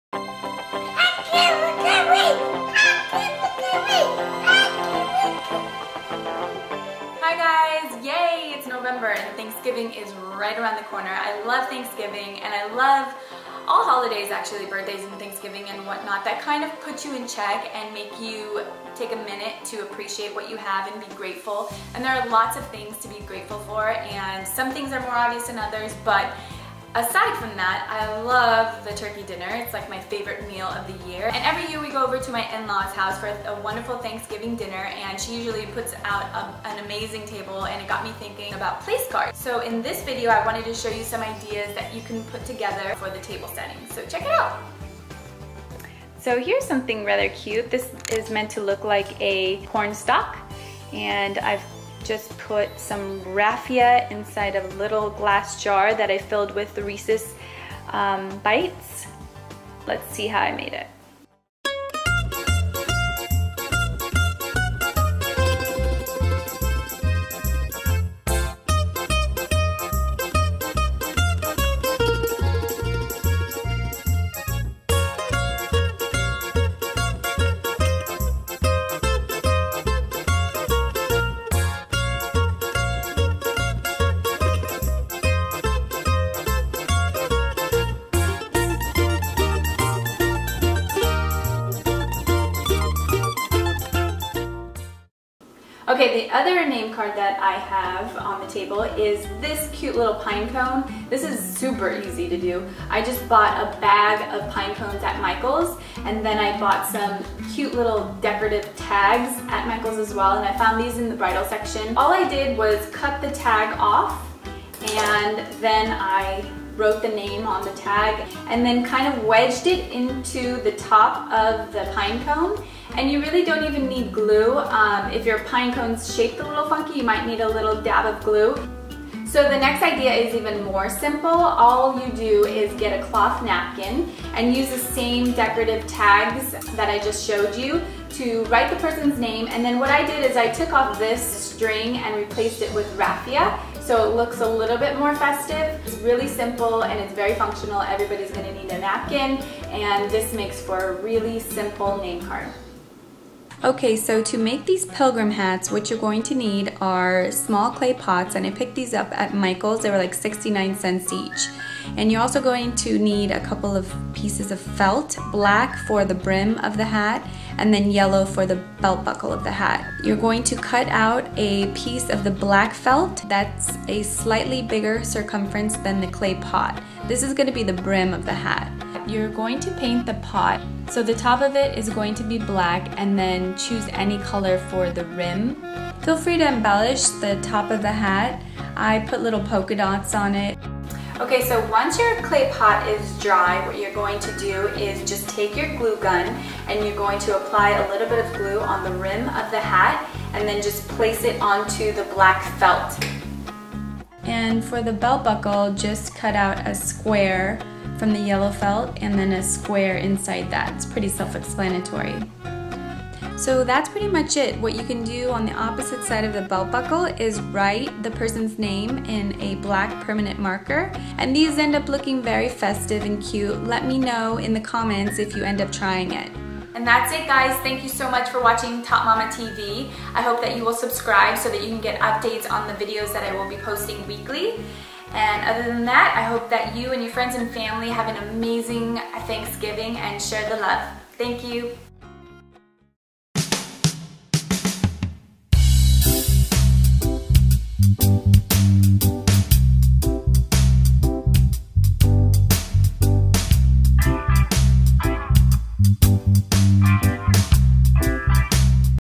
Place cards for Thanksgiving
Thanksgiving is right around the corner, YAY!!!  Here are some great ideas I've come across for name cards or place cards.  Some of these would be perfect for Thanksgiving, the Holidays, or a dinner party.  I hope you enjoy my video and please fee free to comment.  Thank you so much...Happy Healthy Thanksgiving.